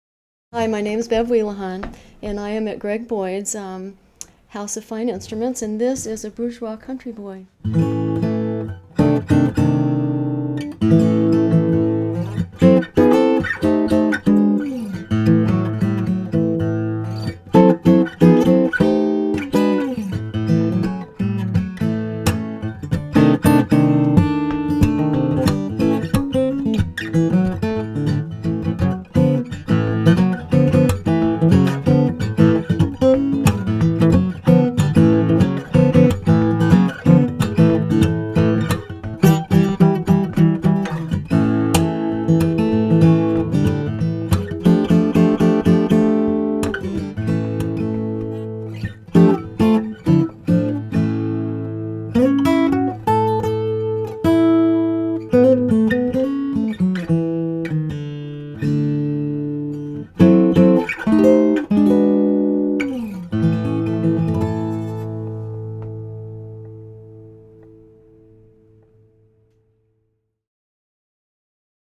2004 Bourgeois Country Boy DeLuxe with Fossilized Mammoth Ivory Bridge
Near mint condition. As-new, very beautiful guitar. 1 47/64" nut width.      Quilted mahogany back and sides. Highly figured bearclaw Adirondack Red Spruce top. Wonderful, straight-grain mahogany neck. Brazilian Rosewood peghead overlay. Gabon ebony fingerboard with small dot inlays. Bourgeois logo inlaid at top of peghead. Solid fossilized mammoth ivory bridge. *Ivory bridges are no longer available from Bourgeois. When they were available, it was a $1000 option. Bone bridge pins with black dot in center. "Tor-tis" pickguard. Ebony center strip in back. Strap button professionally installed in heel.      Tone is amazingly great. This is a special instrument that can't even be ordered today, as Bourgeois no longer offers the ivory bridge option. This guitar cannot be made today by Bourgeois, but they tell us it would list for $6500 if it were available to order. It sounds great and looks striking.